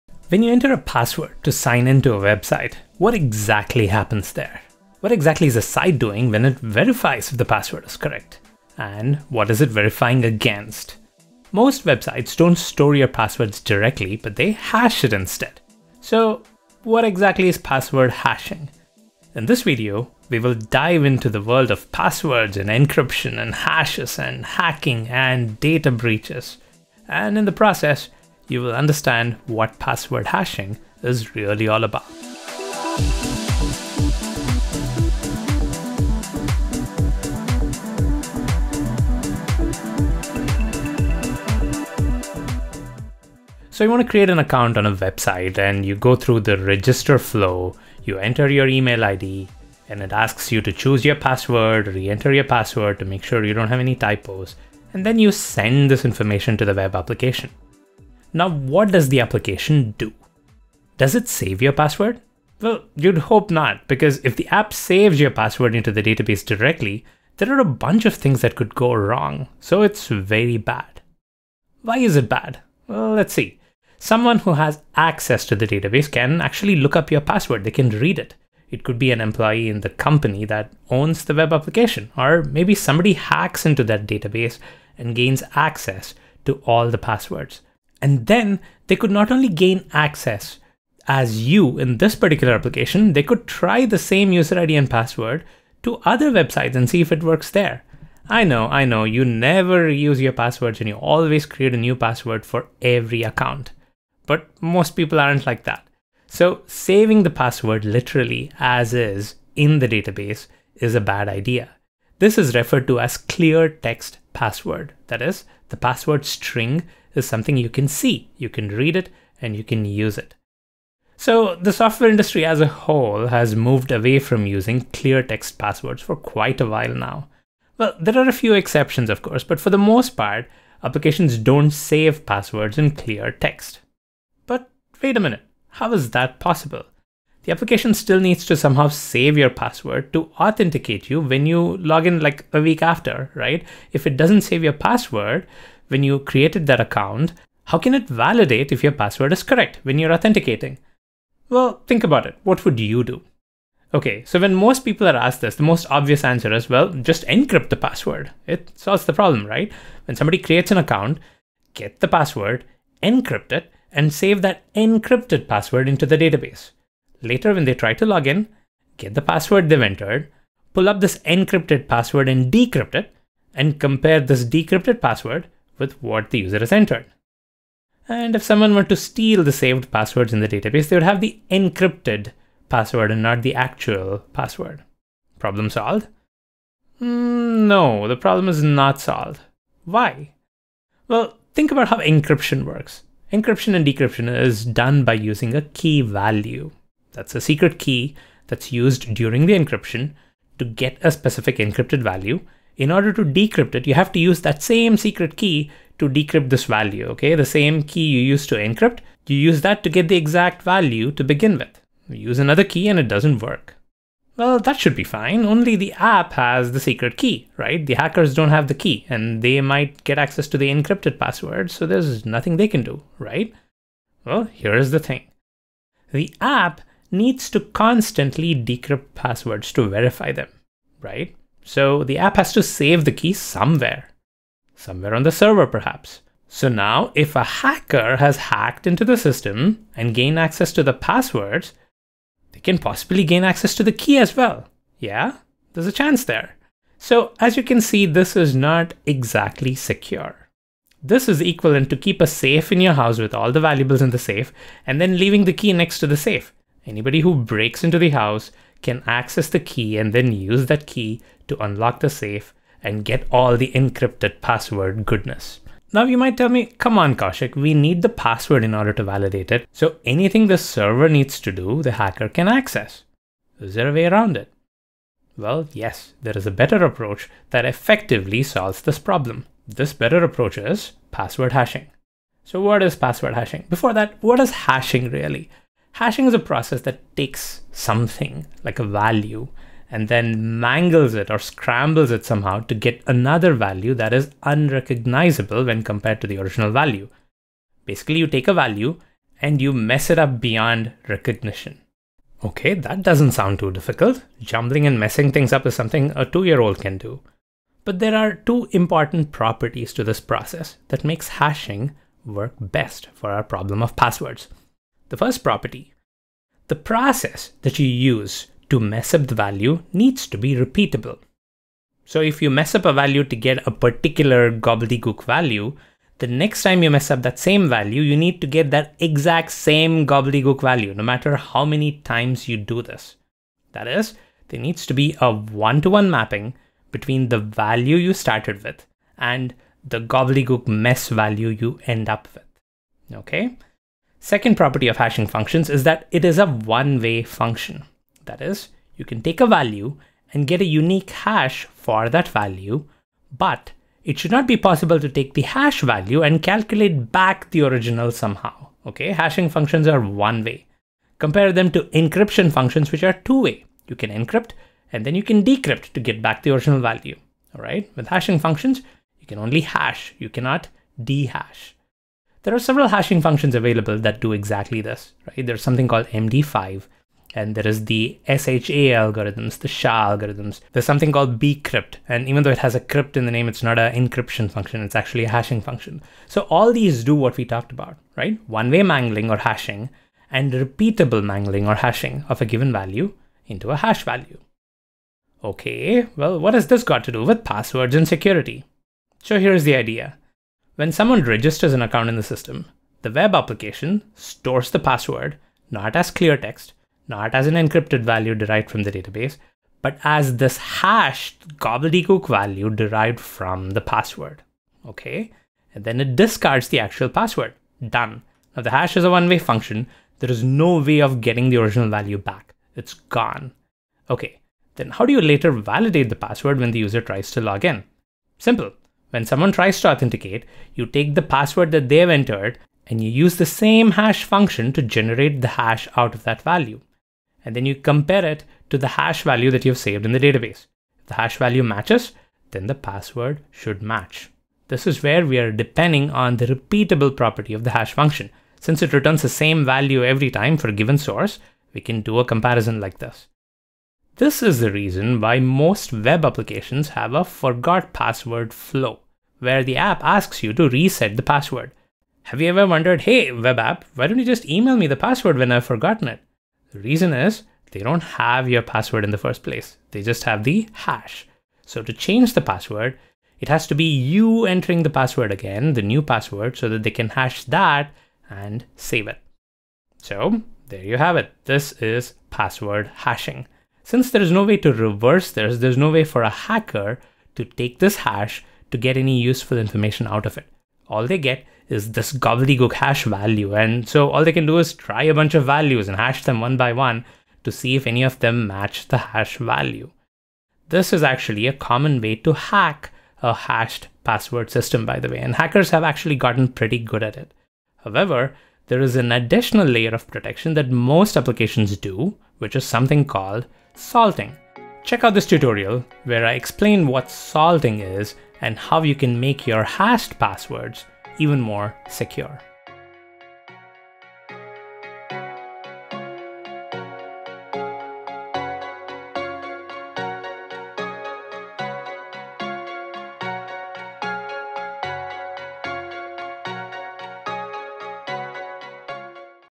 What is password hashing really about - Java Brains
When you enter a password to sign in to a website, what exactly happens there? What exactly is the site doing when it verifies if the password is correct? And what is it verifying against? Most websites don’t store your password directly, but they hash it instead. So, what exactly is password hashing? In this video, we will dive into the world of passwords and encryption and hashes and hacking and data breaches. And you’ll learn what password hashing is really all about.